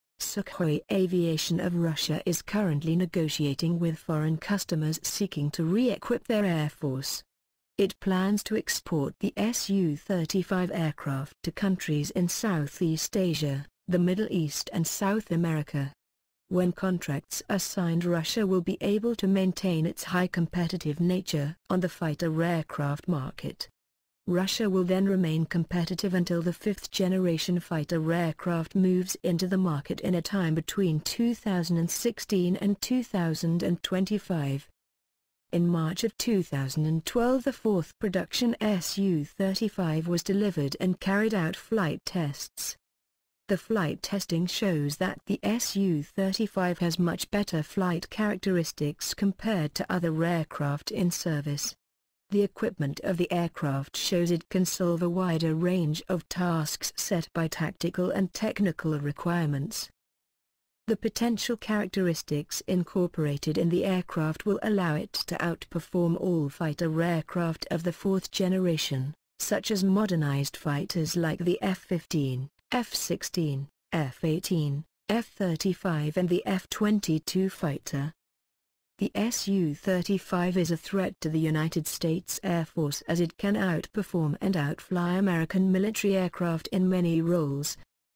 Sukhoi Of Russia To Export The 4th Generation SU-35 Advanced Jet Fighter
- Sukhoi is currently negotiating with foreign customers seeking to re-equip their Air Force. It plans to export Su-35 aircraft to countries in Southeast Asia, the Middle East and South America. If new contracts are signed, Russia will be able to maintain its high competitiveness on the fighter jets world market until the 5th generation fighter aircraft enters the market in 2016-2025.

At present, the Joint State Tests (JST) of the airplane are going on. In March 2012 the fourth production Su-35S was delivered to the JST. The Su-35-1 and 2 carried out preliminary flight tests, during which the main established flight and technical characteristics of the on-board equipment and super maneuverability features were fully confirmed, stability and controllability characteristics, the characteristics of the power plant and the work of the navigation system were tested.

The plane reached the maximum ground-level speed of 1,400 km/h, speed at altitude — 2400 km/h, the ceiling — 18 thousand meters. The detection range of targets in the "air-to-air" mode is over 400 km. This is significantly higher than that of the combat aircraft currently in service. The onboard OLS (optical locator station) can detect and track multiple targets at ranges exceeding 80 km. More than 400 flights were made as part of flight testing on the fighters. The aircraft complex is ready to undergo tests for combat use.

The analysis of the amount of work already done allows a conclusion that Su-35/Su-35S has a much better flight characteristics compared to analogue aircraft in service. The airborne equipment of the plane allows solving a wider range of tasks set by tactical and technical requirements.

The potential characteristics incorporated in the aircraft will allow it to exceed all tactical fighters of the 4th and 4 + generations, such as "Rafale" and EF 2000, modernized fighters like the F-15, F-16, F-18, F-35 and to counter the F-22A fighter.